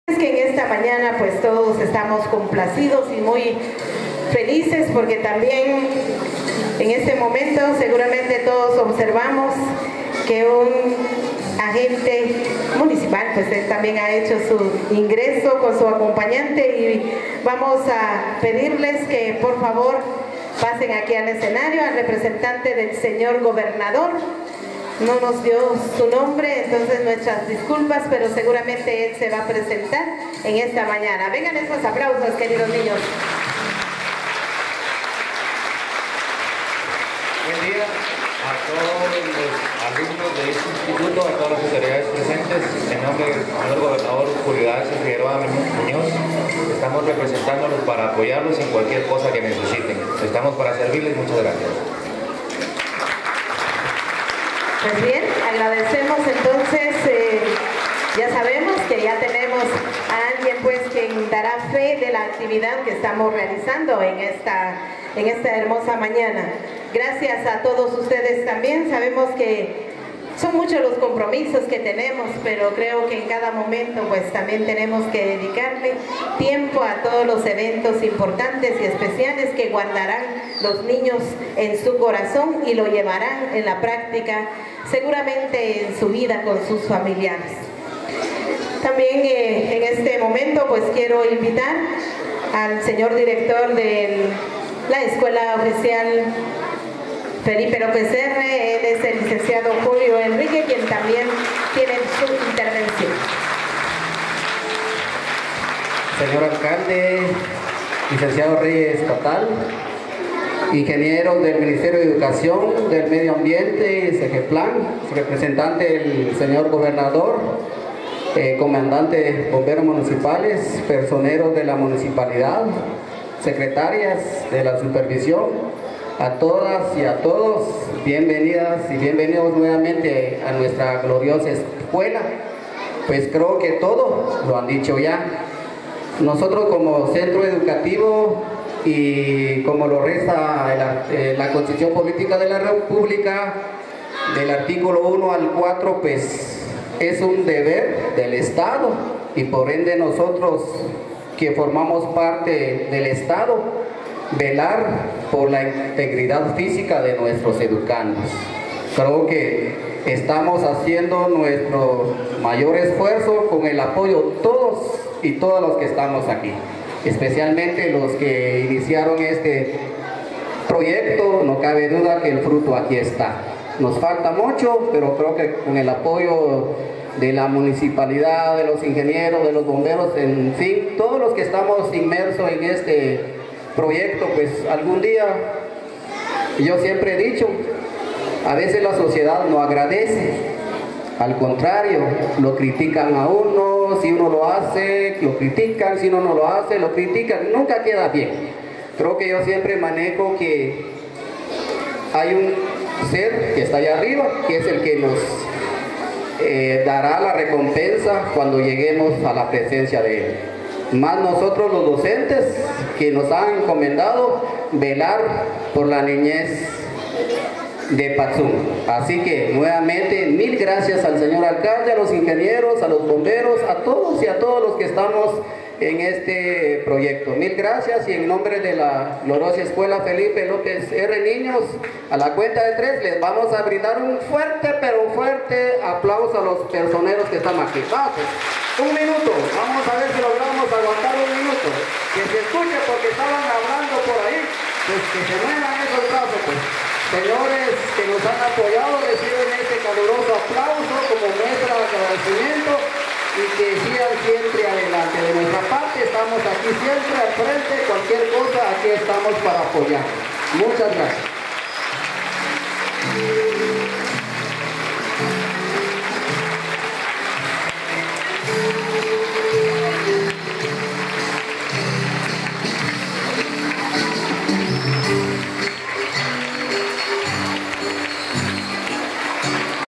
Al cierre de los actos con motivo de simulacro
Patzún, participación del representante del Gobernador Departamental de Chimaltenango y del director Julio Enrique Xinico Cum, al cierre de los actos realizados en la Escuela Oficial Urbana Mixta Integral, Felipe López R. Con motivo del segundo simulacro anual realizado para asumir medidas de prevención en caso de sismos.